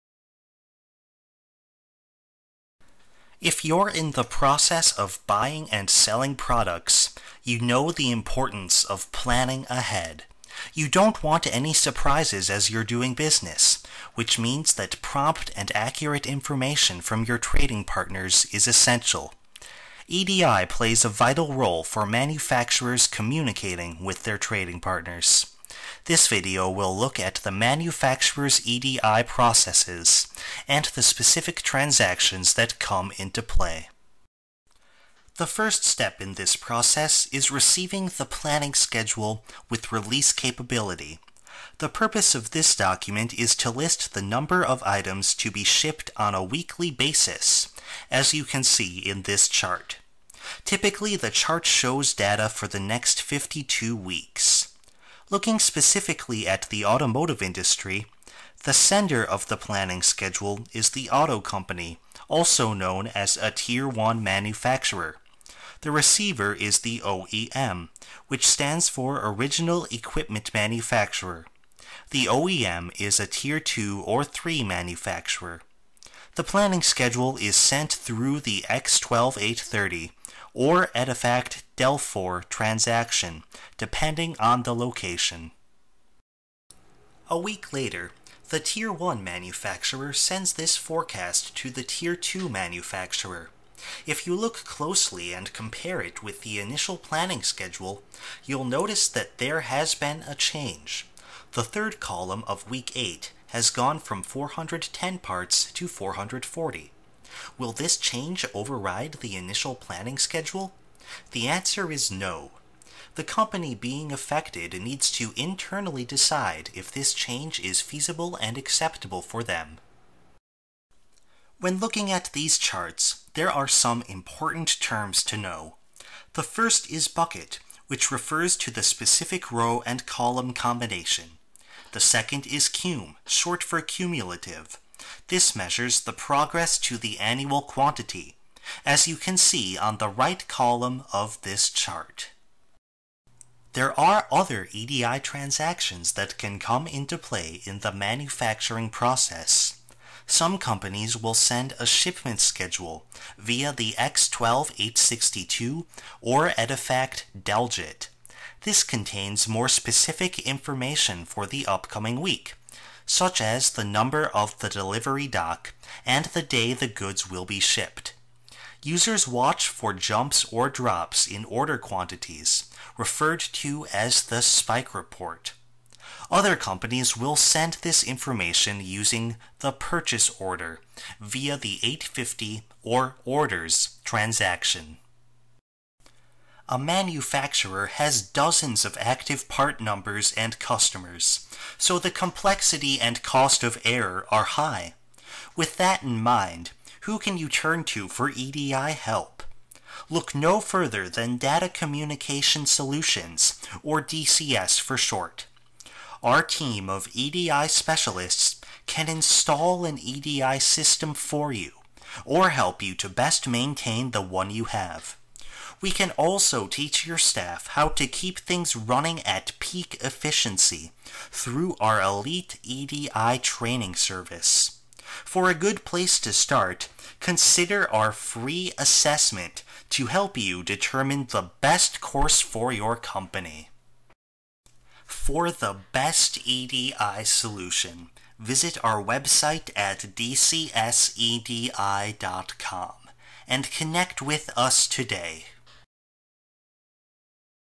Manufacturers' EDI Processes
When it comes to the manufacturing and distribution of products, it's important to plan ahead.  EDI plays a vital role in communication between manufacturers and their trading partners.  This video explains the transactions that come into play during this process.